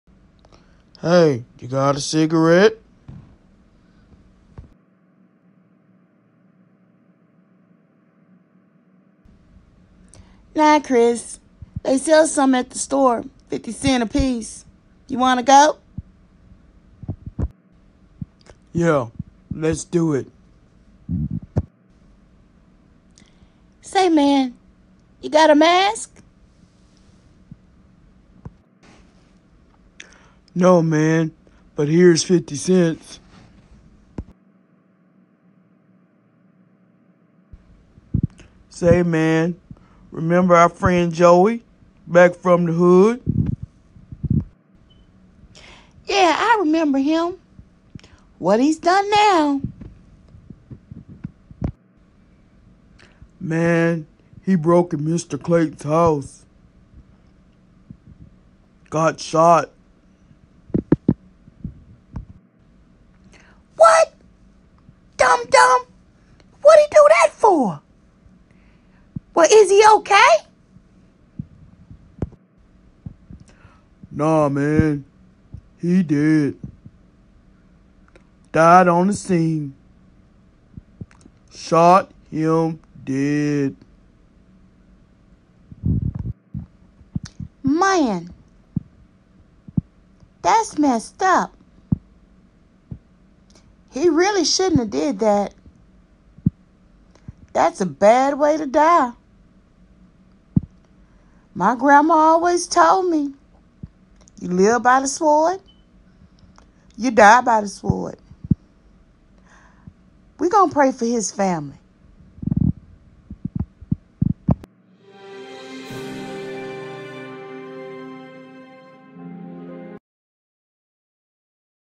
BOYZ N DA HOOD ep 1
Scott and Chris are homies catching up after a few years. What lessons have they learned?

Content created on Plotagon app for iphone